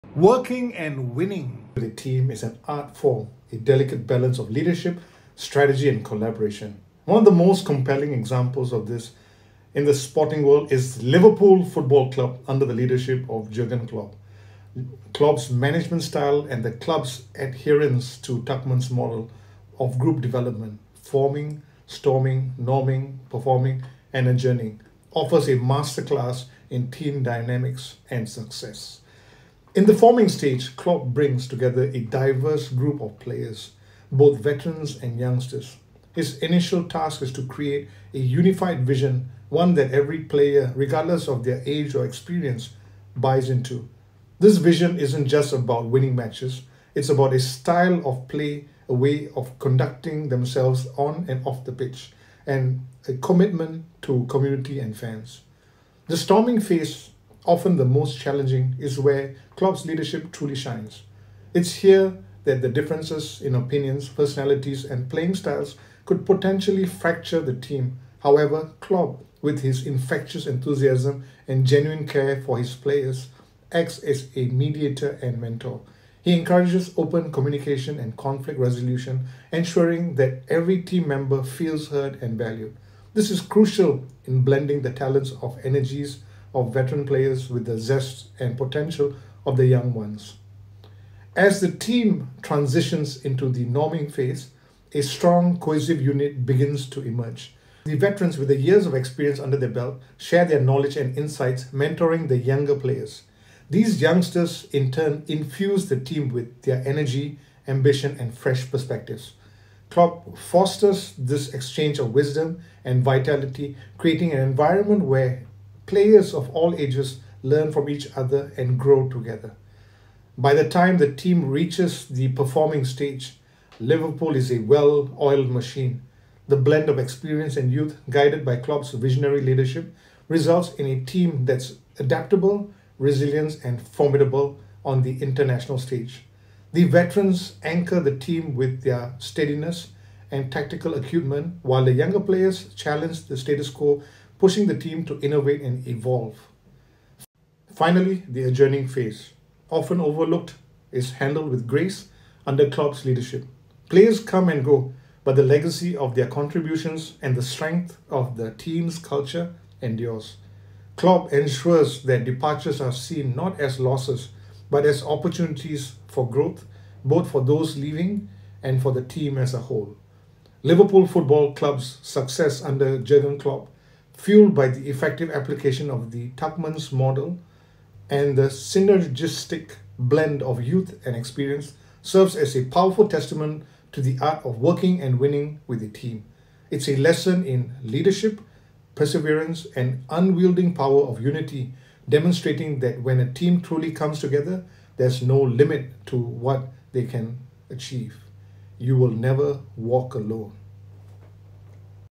Winning Secrets: How Klopp’s Liverpool Unites Young Talents and Veterans for Glory!
Dive into the heart of Liverpool Football Club's success story under the legendary Jürgen Klopp! This video explores the genius of Klopp’s leadership and the application of Tuckman's model in blending the raw energy of young talents with the seasoned expertise of veteran players. From the training ground to the electrifying atmosphere of Anfield, discover the strategies that make Liverpool not just a team, but a family bound for victory. Whether you're a die-hard Liverpool fan, a soccer enthusiast, or someone interested in team dynamics and leadership, this video is for you! Join us as we analyze the key elements of Liverpool's triumphs, the mentorship among players, and the unparalleled spirit that drives the team forward.